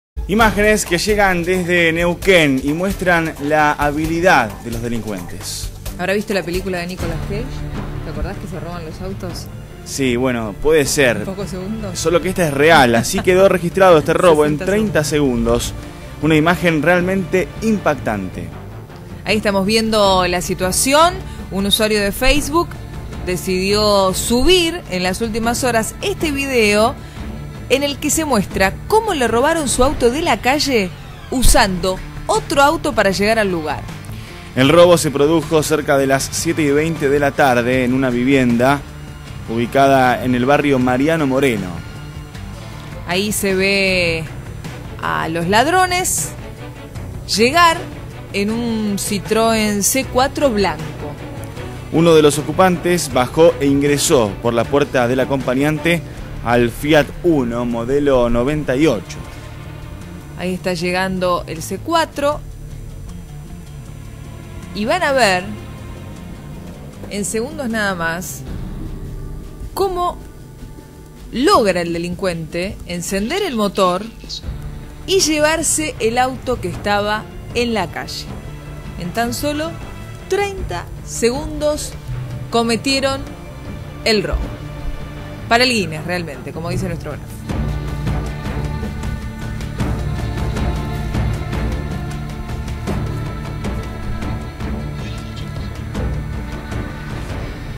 Canal 26 - INCREÍBLE ROBO A VEHÍCULO
En Neuquén roban un auto en solo 30 segundos. Aquí las imágenes.